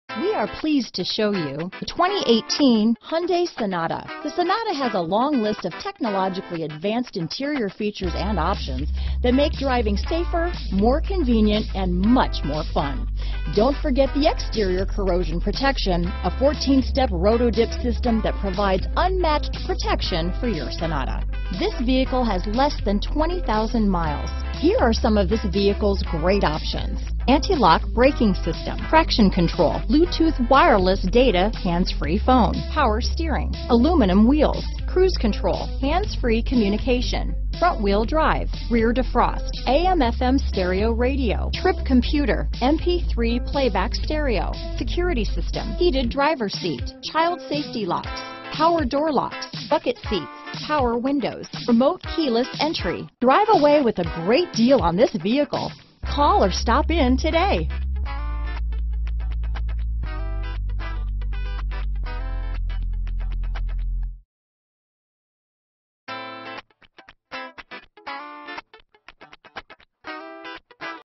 2018 Hyundai Sonata Ontario, Claremont, Puente Hills, City of Industry, Inland Empire, CA P632425
Used 2018 Hyundai Sonata available in Puente Hills, California at Puente Hills Hyundai. Servicing the Ontario, Claremont, City of Industry, Inland Empire, CA area.

SEL WITH TECH PKG ITS LOADED! HUGE DIFFERENCE THIS ONE COMES WITH REMAINING HYUNDAI 10YR/100K MILES WARRANTY FOR PEACE OF MIND PLUS THE CAR IS LIKE NEW AND SMELLS LIKE NEW! COME SEE IT TODAY!  brBackup Camera Bluetooth / iPhone Integration Lane Assist / Lane Departure Smart Cruise Control / Active Braking / Distance Control Automatic Emergency Braking (AEB) Electronic Parking Brake (EPB) Lane Keep Assist System (LKA) Option Group 02 Smart Cruise Control w/Start/Stop Tech Package 02. Clean CARFAX. 25/35 City/Highway MPGbrbrbrPuente Hills Hyundai ...Why Go Anywhere Else!